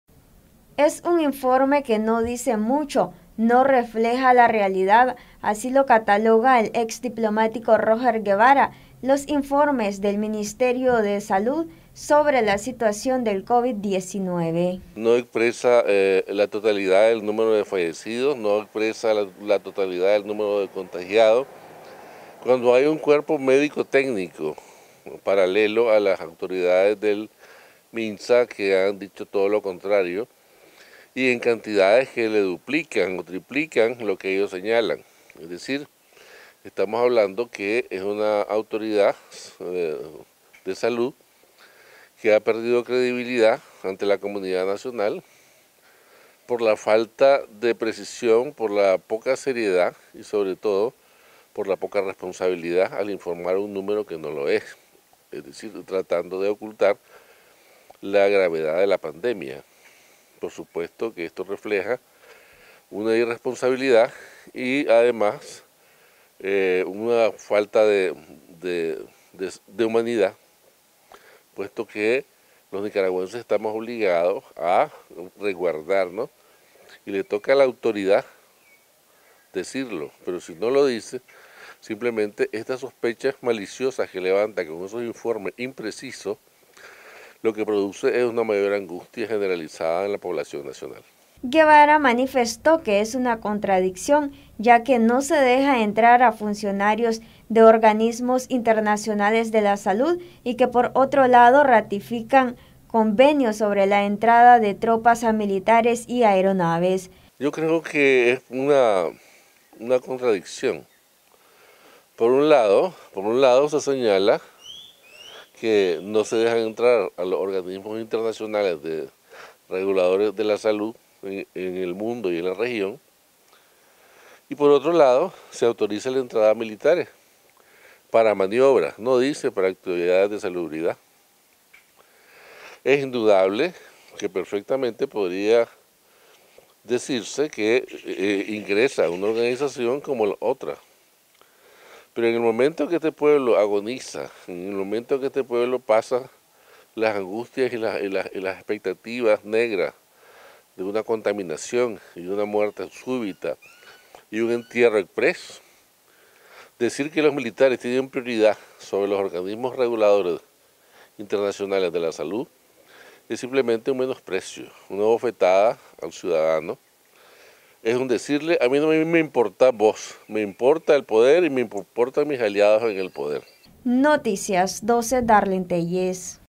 “Informes del Minsa no expresa la totalidad de fallecidos por coronavirus”
Informes del Ministerio de Salud no reflejan la realidad afirma el ex diplomático Roger Guevara.